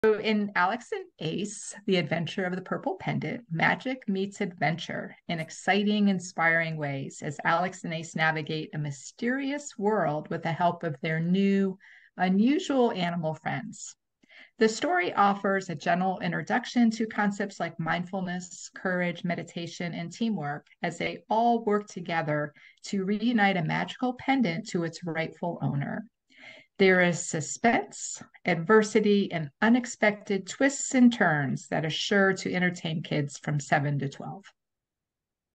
Reviewing Alex & Ace | The Adventure of the Purple Pendant by Susan Steinman
Middle-grade children's author, RN, and master of Science & Nursing, Susan Steinman shares her review of the first book in the Alex & Ace series: The Adventure of the Purple Pendant